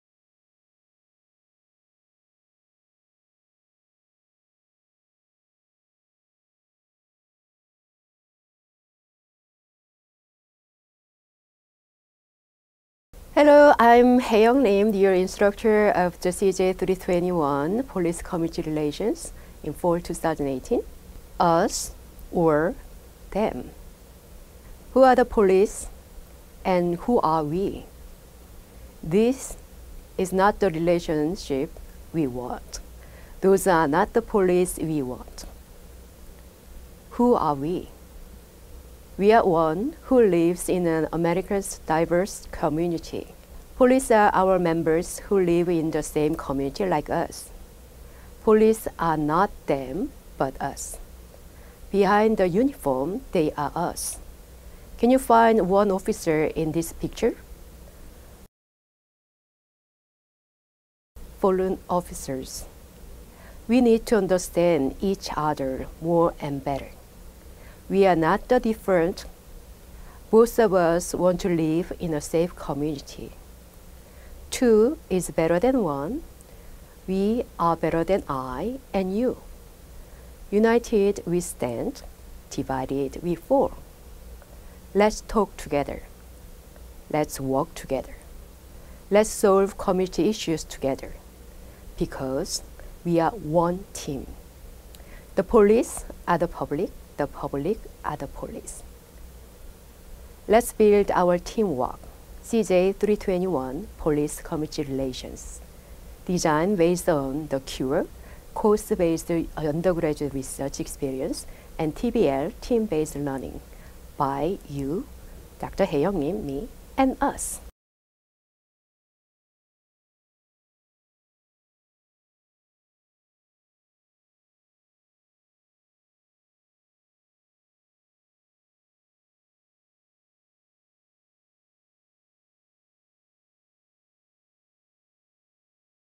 Us or Them?
This is a promotional video for a new course offering in the Department of Criminal Justice at UAB.